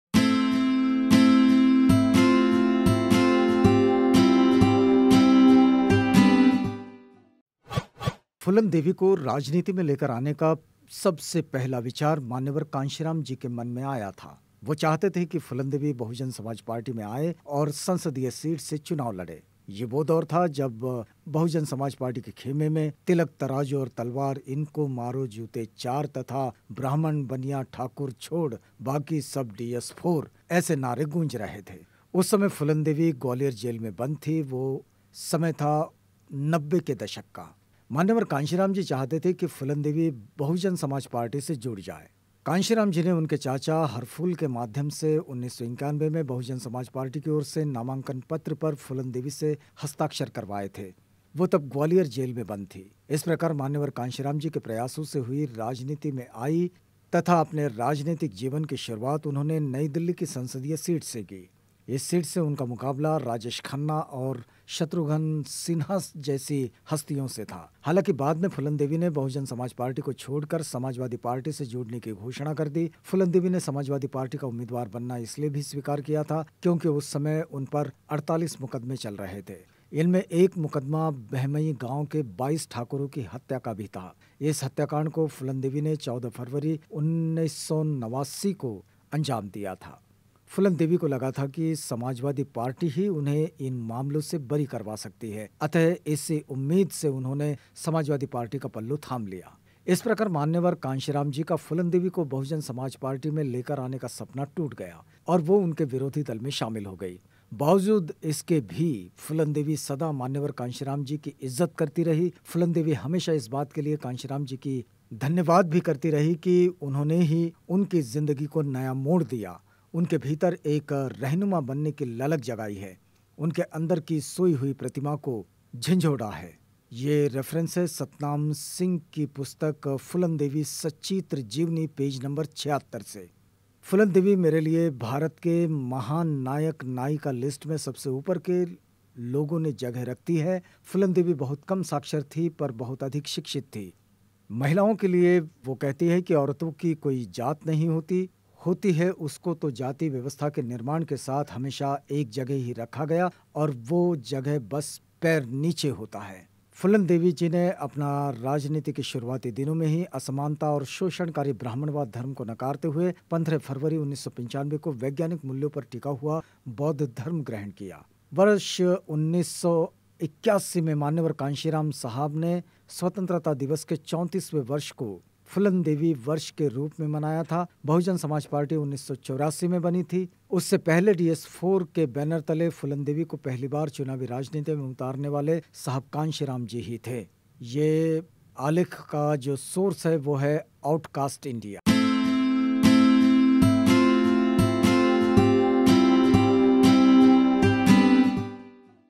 मान्यवर कांशीराम ने फूलन देवी को जब पहली बार BSP का उम्मीदवार बनाया था Phoolan Devi : Kanshiram BSP
Phoolan devi contested on BSP Ticket from Delhi against Rajesh Khanna & Shatrughan Sinha